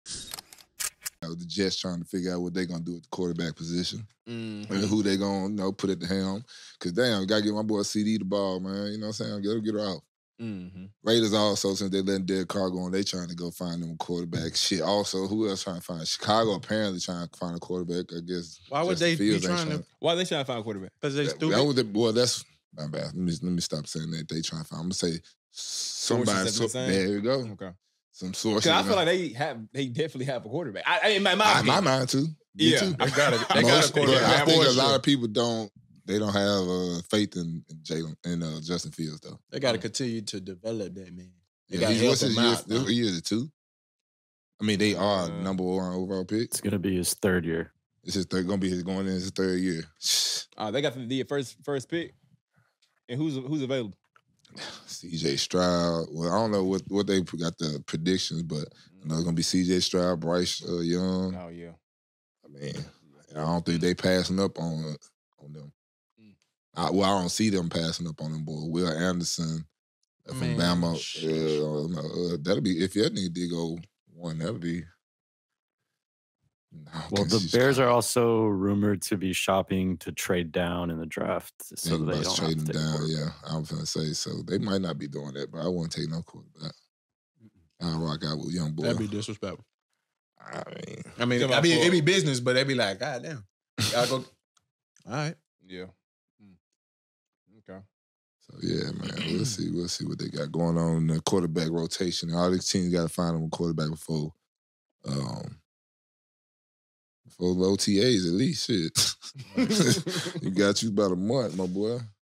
NFL Veterans Discuss If Bears Should Trade First Pick in NFL Draft
NFL veterans Daren Bates & Jalen Collins discuss with former Memphis Tigers track standout Alex Sweet about the Bears options in the NFL Draft with the first choice. Taken from Raw Room episode "Bonzai Tree"

Produced & Edited by:
Feyzan Shareef

Social Media Manager/Graphics:
Matt Keaton:

Fulfillment Operations Intern:
Jon Maine: